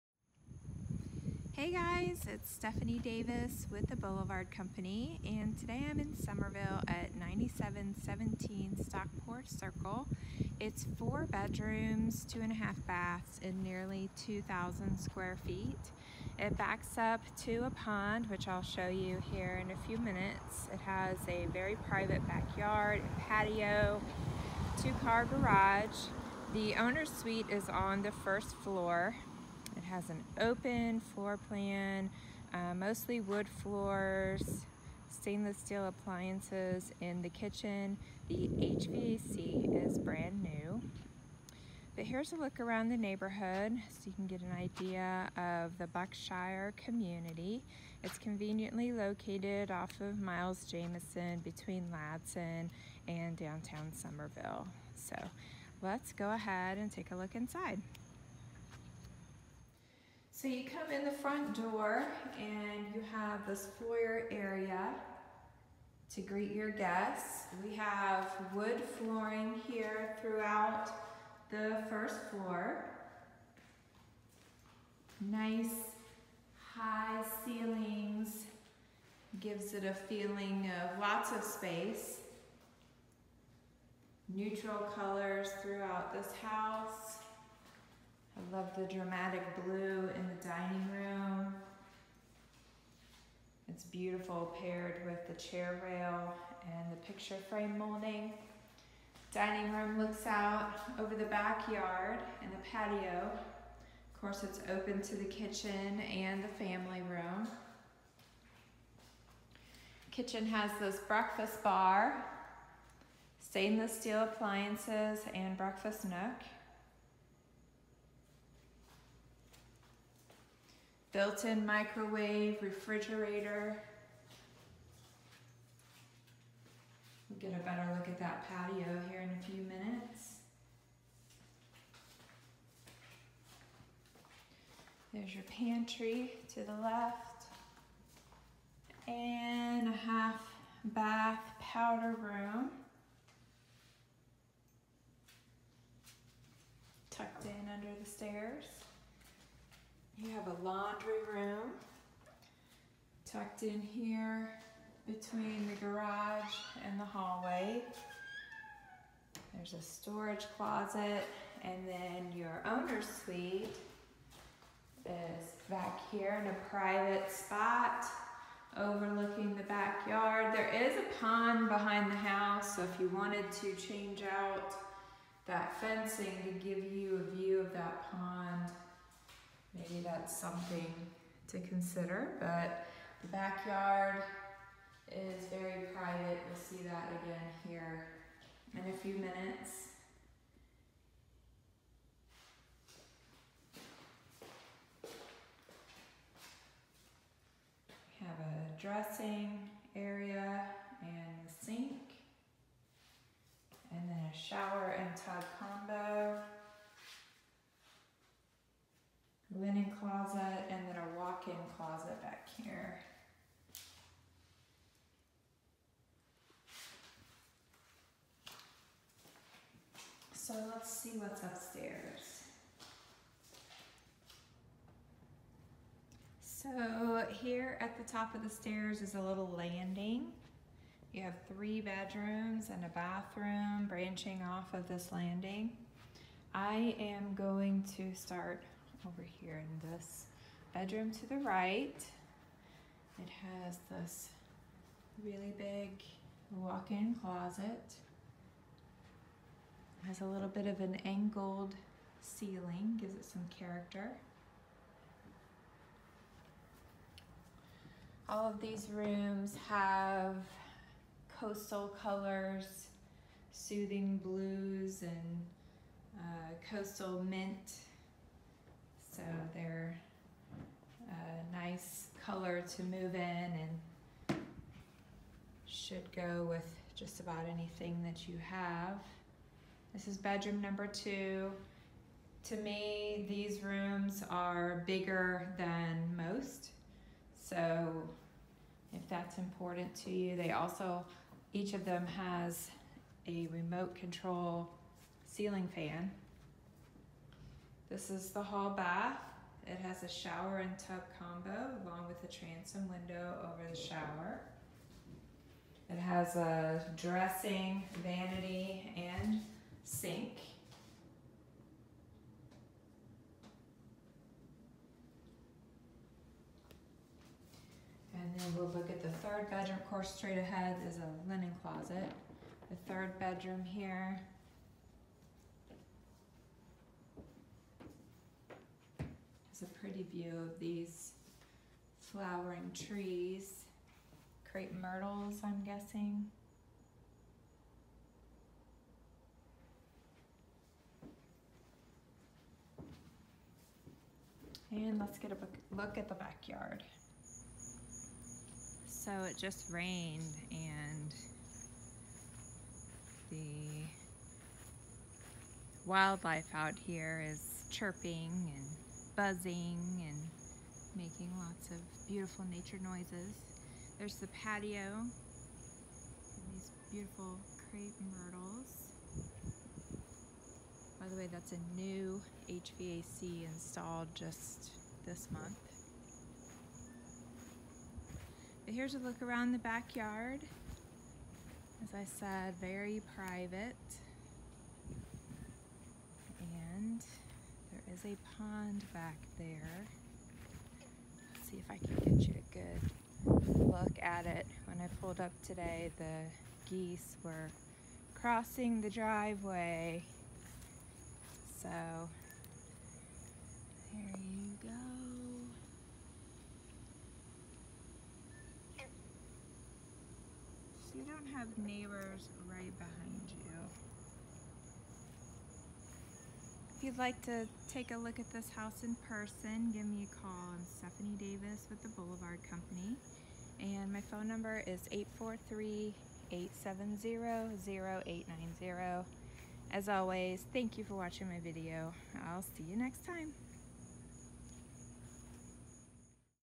SOLD :: 4 BR House for Sale in Summerville, SC :: 9717 Stockport Circle in Buckshire
SOLD!

This 4BR, 2.5 BA home is just 4.5 miles to downtown Summerville, and 5.5 miles to I-26. It backs to a pond for privacy, and offers a brand new HVAC, PLUS a new water heater. The first floor is open concept, featuring an eat-in kitchen with a breakfast nook that overlooks the patio and backyard; a separate dining room dressed up with crown molding and picture frame wainscoting; and a 2-story family room. The owner's suite is on the first floor and has new flooring (2018), a private bathroom with soaking tub and separate shower, and a walk-in closet. The laundry room and a powder room complete the first level. Upstairs, there are three spacious bedrooms and a hall bathroom. The grilling patio is very private, and situated under a beautiful shade tree. The backyard is fully enclosed with a privacy fence. The 2-car garage has an electric door opener. Buckshire has a community pool, and is centrally located for commuters, with close proximity to the interstate, Boeing, downtown Summerville, and Volvo.